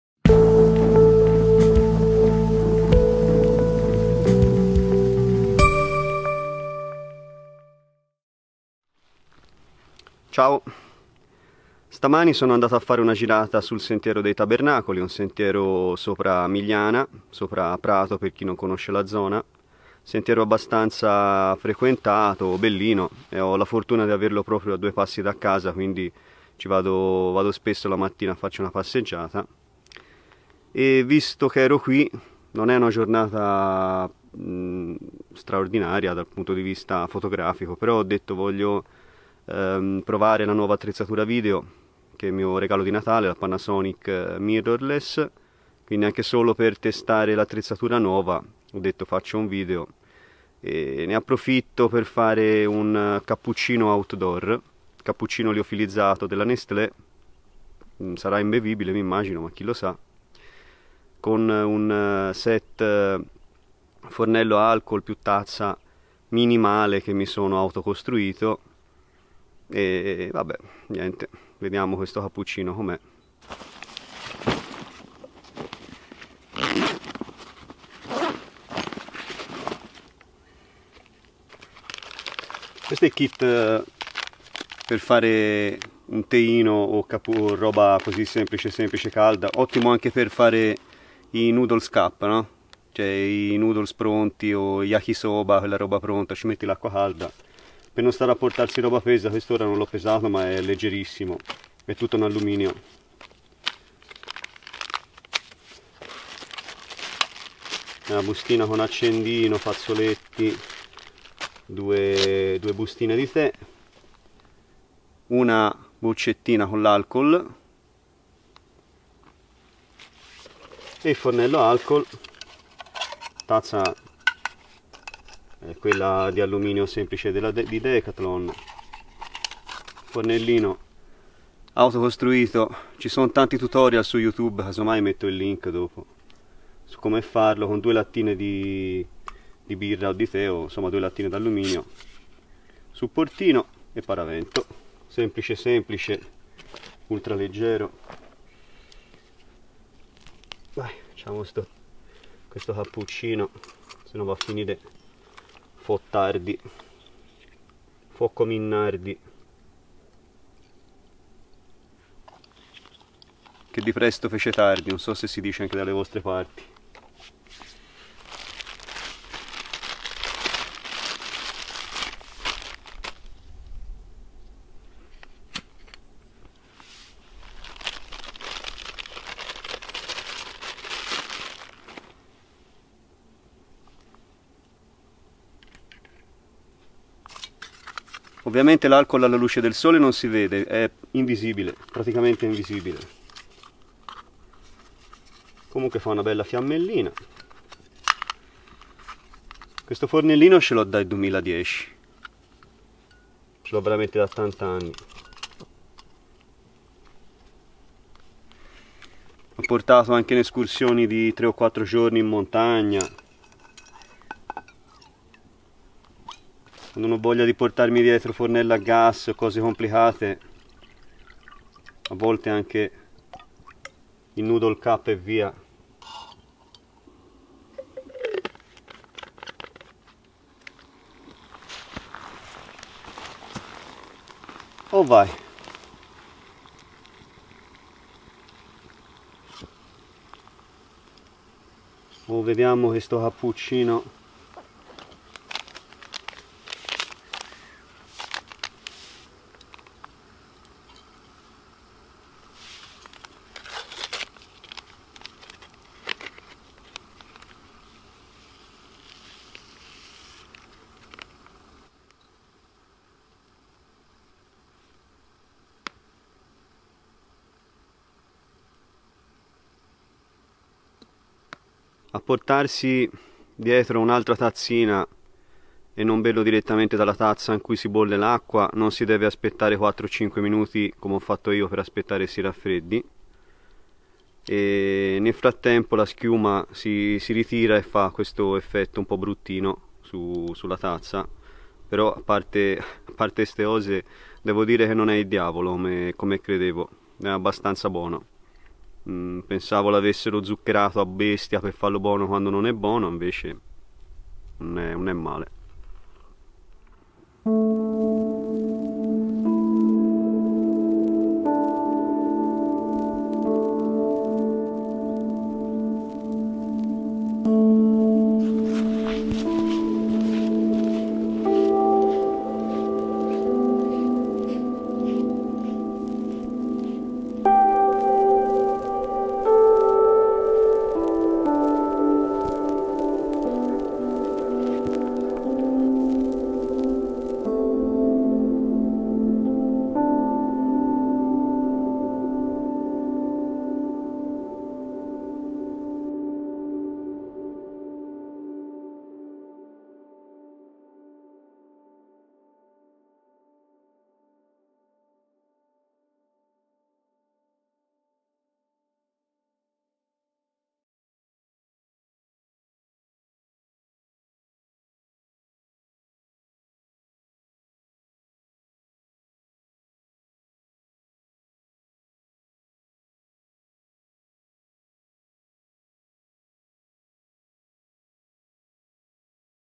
Misc: Cappuccino Outdoor
60 . Cappuccino liofilizzato outdoor mentre testo il nuovo balocco di natale. Non tremendo come pensavo comunque.

Tutorial per realizzare un fornello ad alcool con due lattine di alluminio. Io usai il tutorial del leggendario StefanoBi che purtroppo adesso non è più su YouTube.